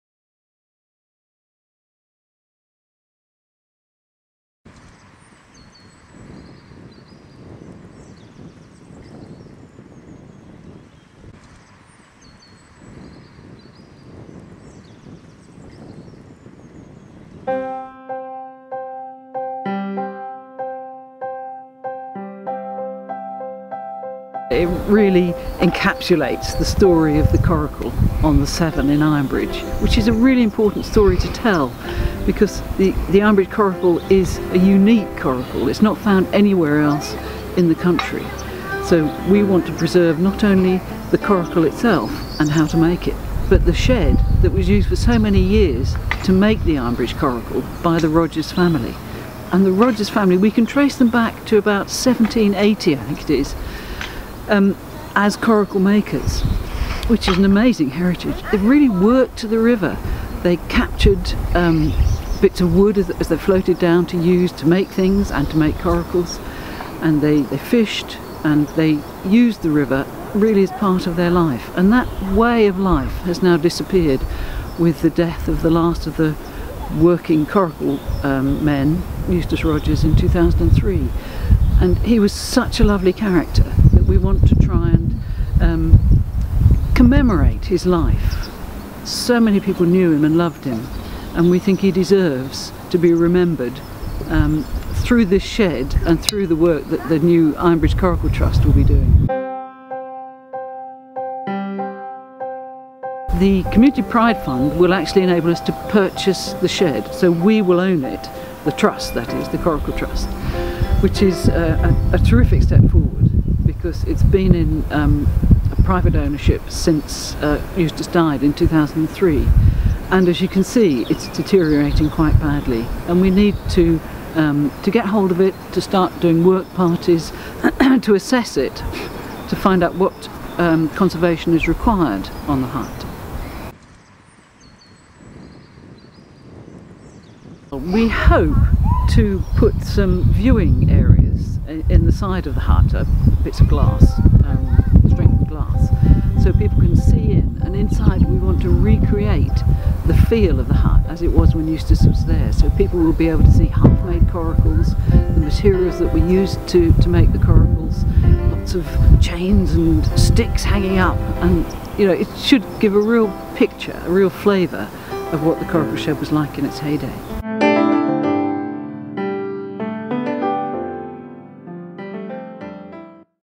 Ironbridge Coracle Trust
A little piece of history is to be saved thanks to a grant of £40,000 from Telford & Wrekin Council's Community Pride Fund. Eustace Rogers was 'The Coracle Man'. On the banks of the River Severn at Ironbridge, he fashioned boats to a design that dates back thousands of years. Now, with the grant, the shed where he manufactured the boats will be bought by The Ironbridge CoracleTrust.